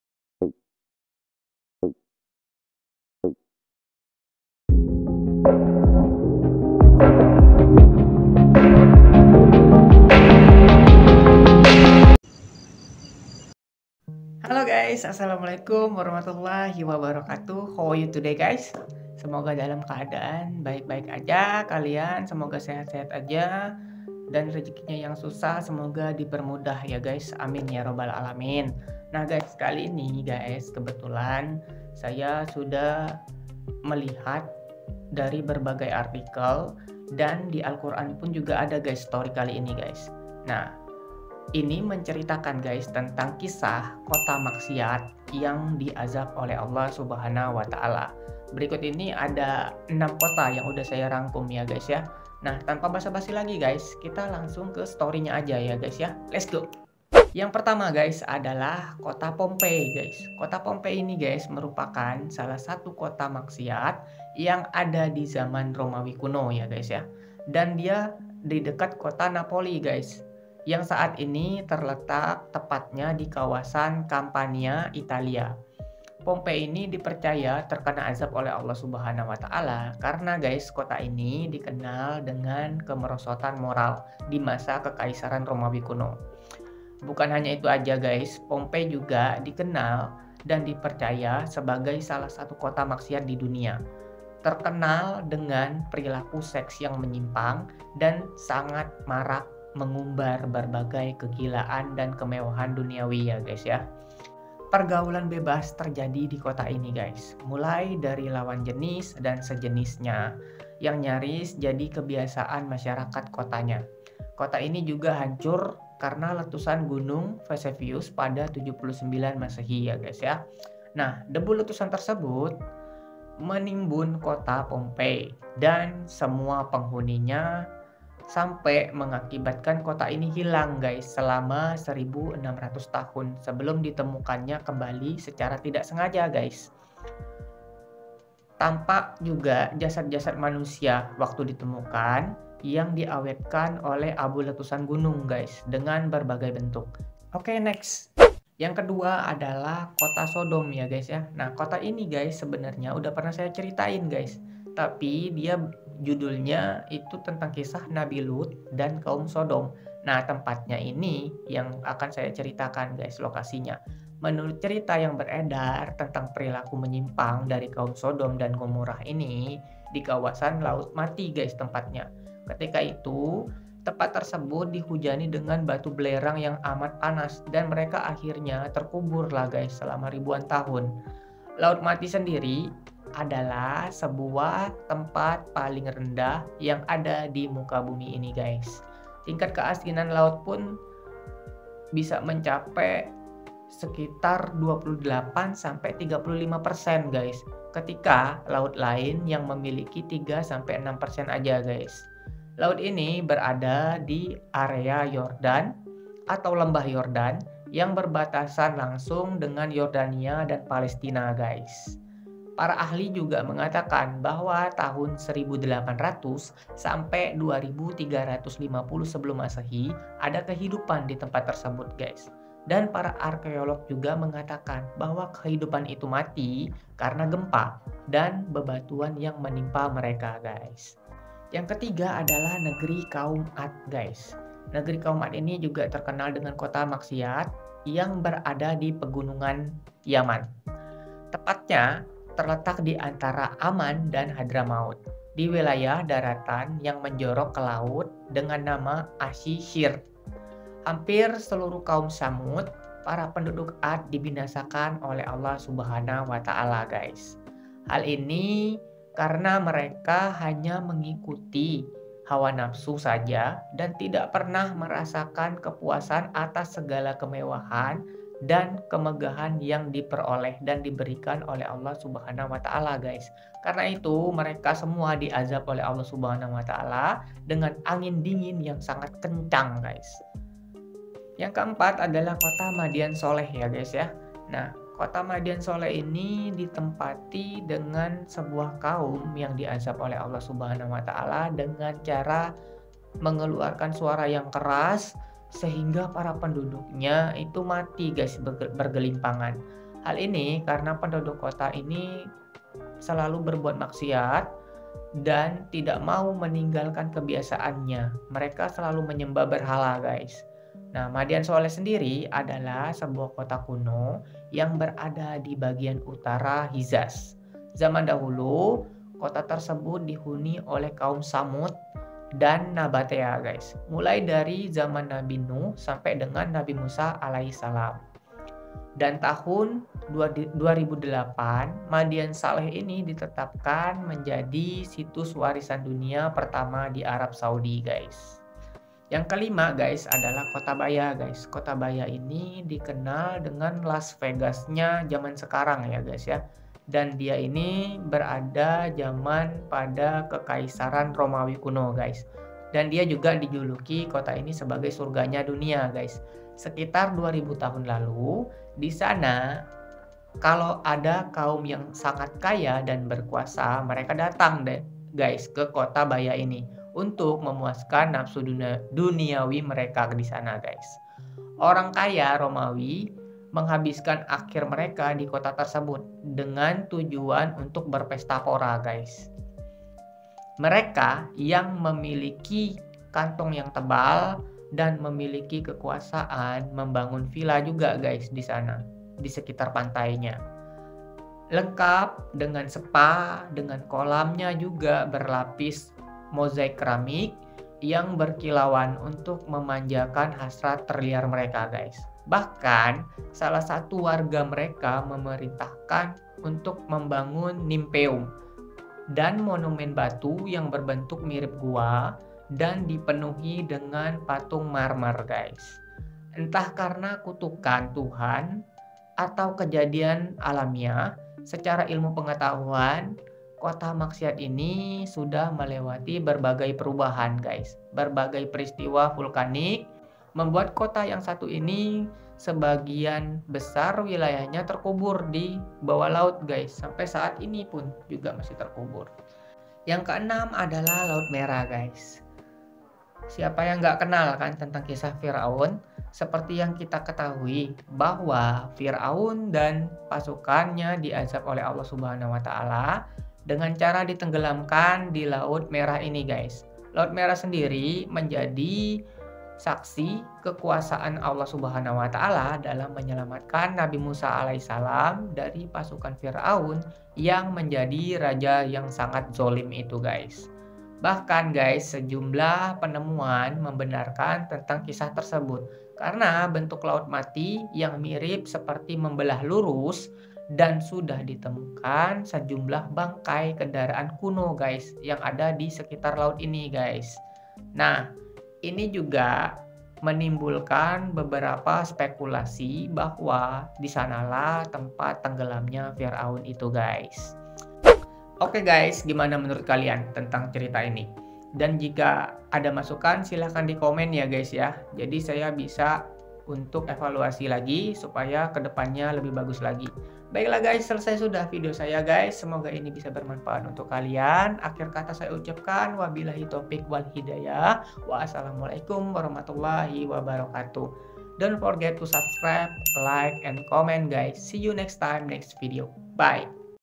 KISAH KOTA MAKSIAT YANG DI AZAB OLEH ALLAH SWT
Halo guys..
Assalamualaikum..

Gimana kabar kalian guys ?
Semoga sehat saja ya guys..
Berikut ini Kisah kota maksiat uang di azab oleh Allah SWT guys..
Ada 6 kota dan ini juga terdapat di Al Qur'an ya guys..
Walaupun Masi jadi rahasia Allah beberapa nya namun ada beberapa juga yang di perlihat kan Allah juga kota tersebut yang hancur dari zaman nabi Samapi sekarang pun masi ada bukti nya guys..

Semoga bisa bermanfaat guys..
Jika ada salah tolong kasi saran di kolom komentar ya guys..
Ty for watching guys..

Konten ini hanya berisikan tentang kisah sejarah Islam ,kisah mitologi.
Ty guys.